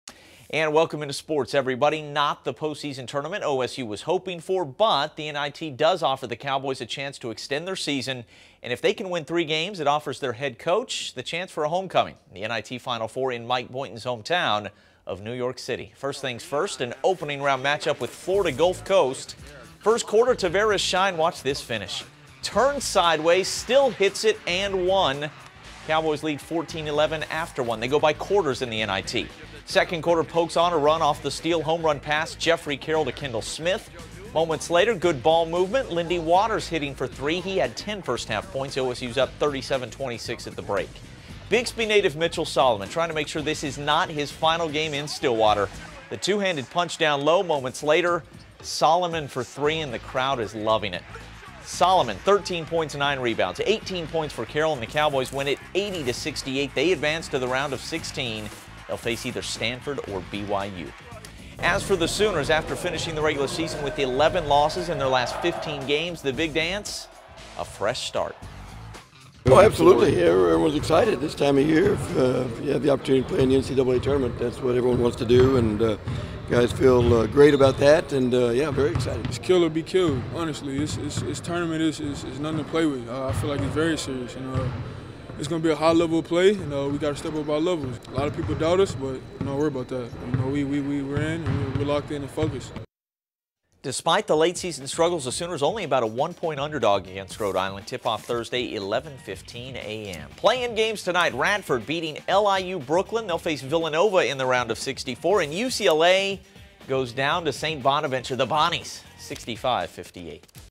Oklahoma State takes down Florida Gulf Coast, 80-68 in NIT opener
Mitchell Solomon scores 13 points and grabs 9 rebounds as Oklahoma State extends its season with an 80-68 defeat of Florida Gulf Coast in round one of the NIT